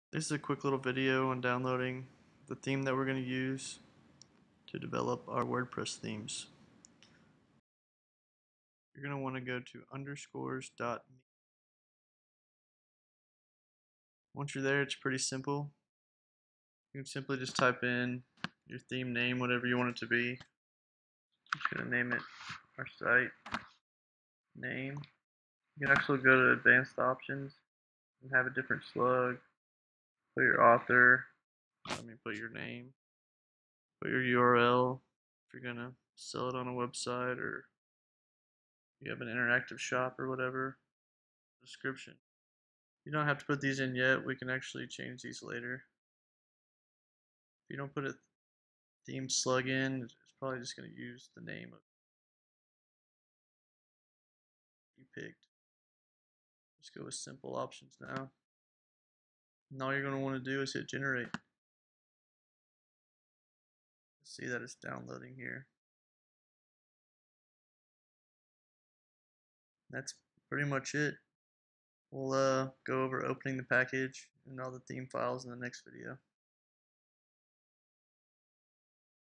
Downloading Underscores WordPress Theme
Quick video on how to download the Underscores.me WordPress theme. View more tutorial on bootstrapwp.com